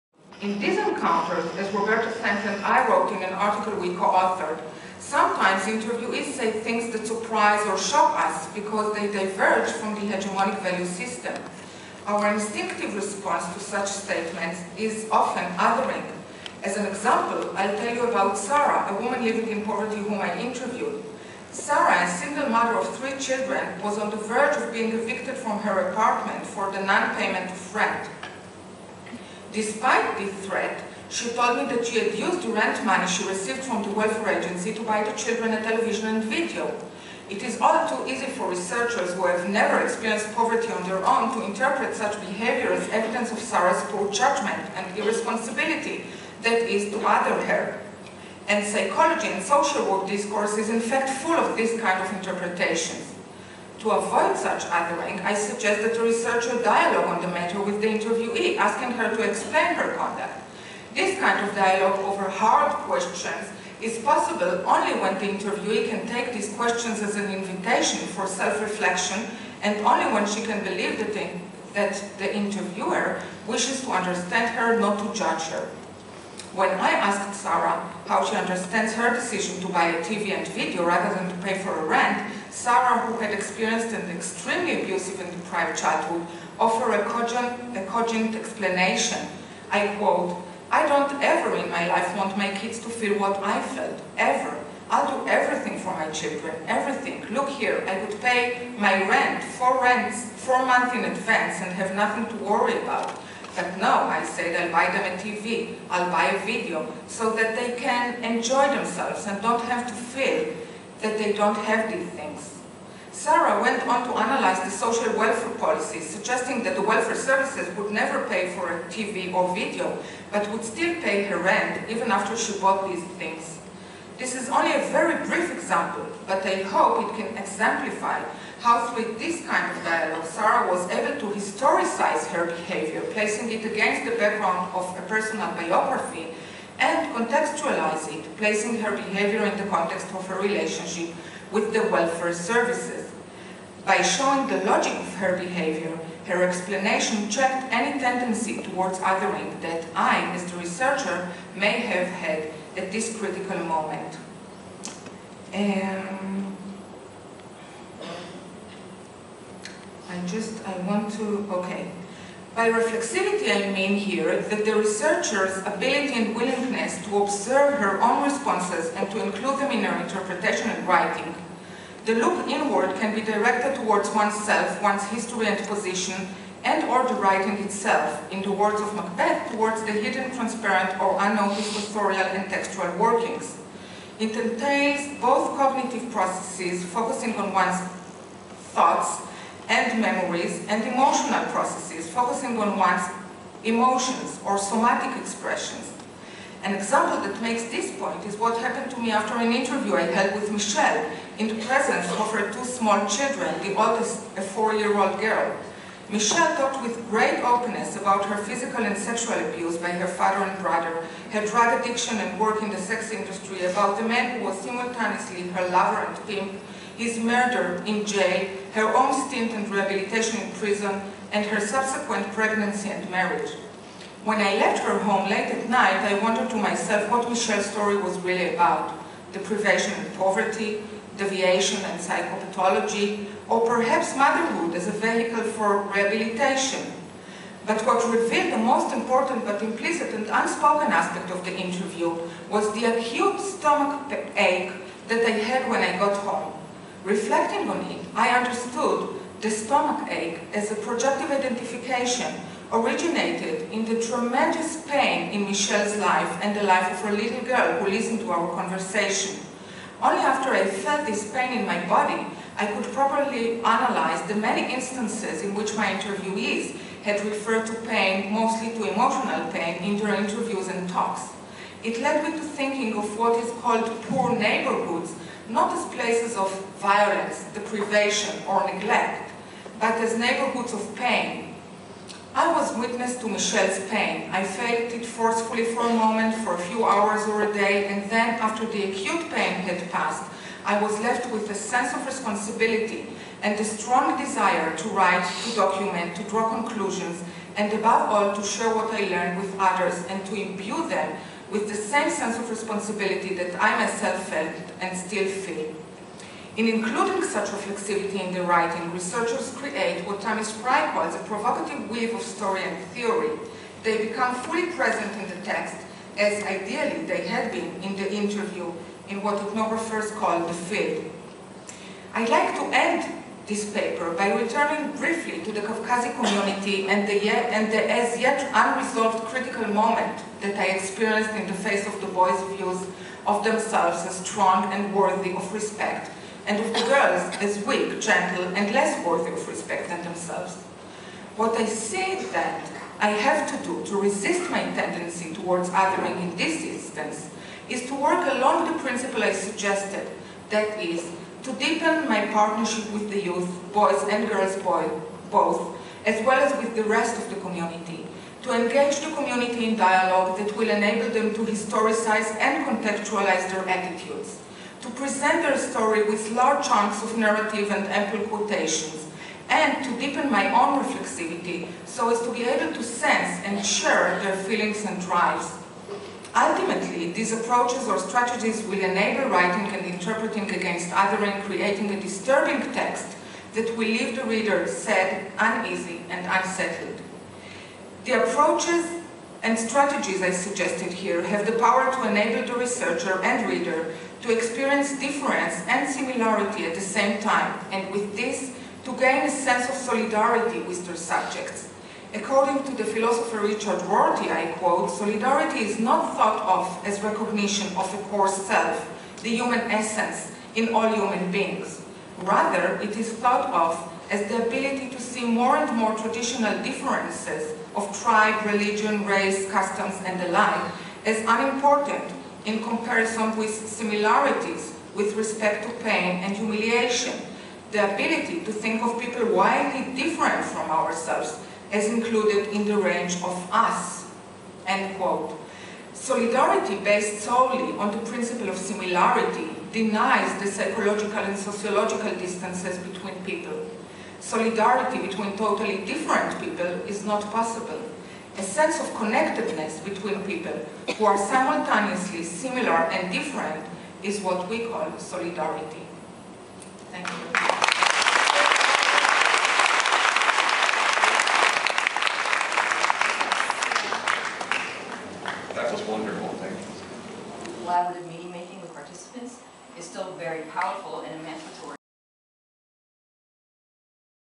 Michal Krumer-Nevo, Writing Against Othering Keynote Speech, Part 2
This is part 2 of a keynote speech on Writing Against Othering by social work researcher Michal Krumer-Nevo, a professor at Ben Gurion University of the Negev. Professor Krumer-Nevo gave the speech at the 7th Annual International Congress on Qualitative Inquiry.  Other videos of ICQI7 are on youtube.